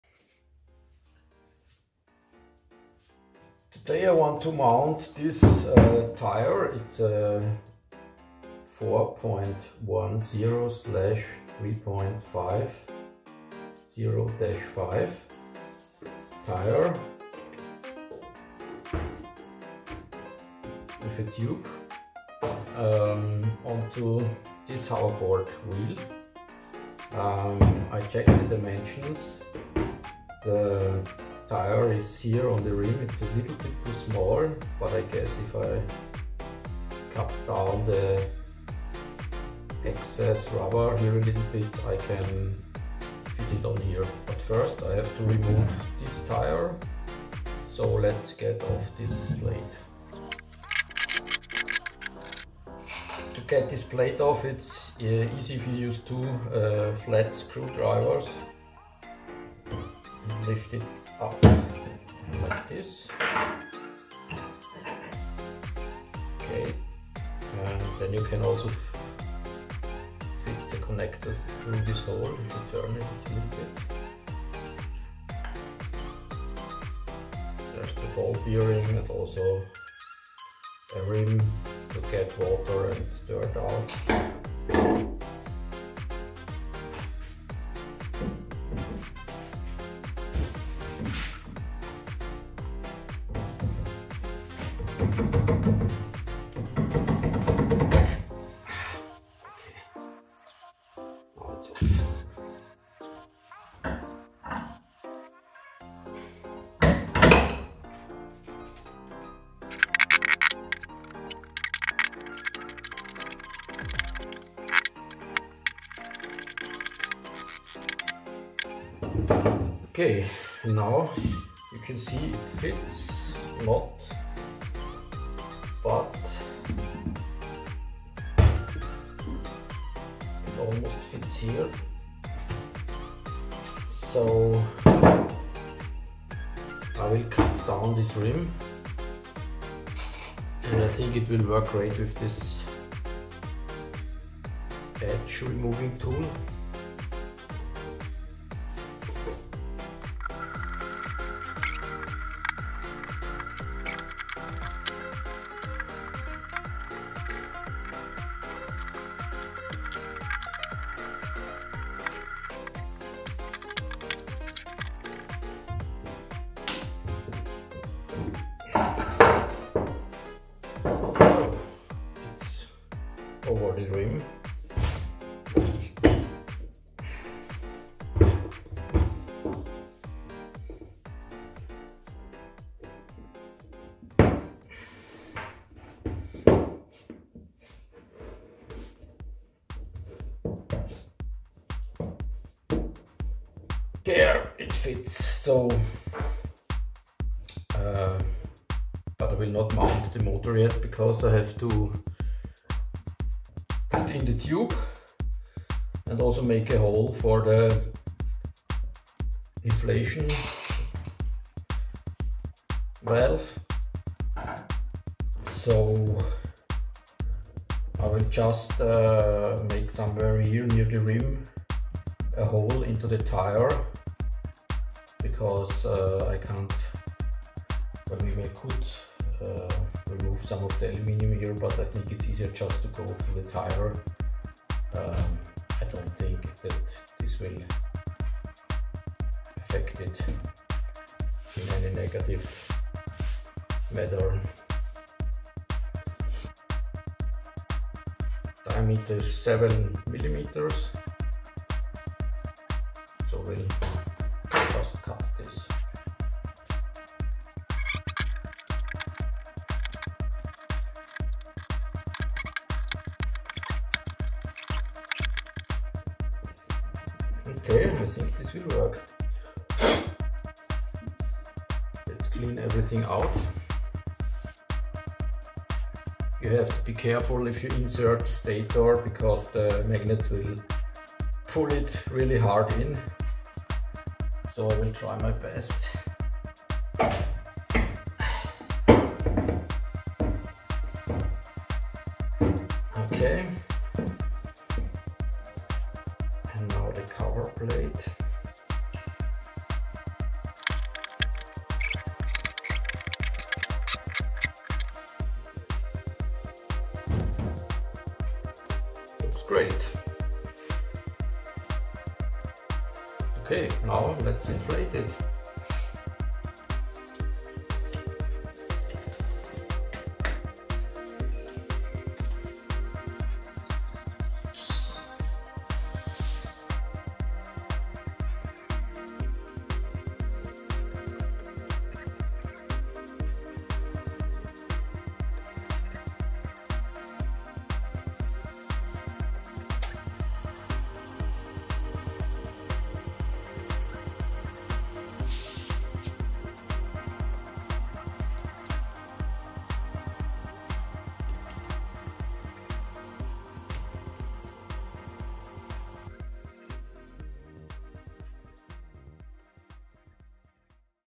Hoverboard Rubber Tire Mounting
New E-Traktor build with Hoverboard Motors - Part 3
Second Kid also wants an E-Traktor / E-Quad.

Deburrer Tool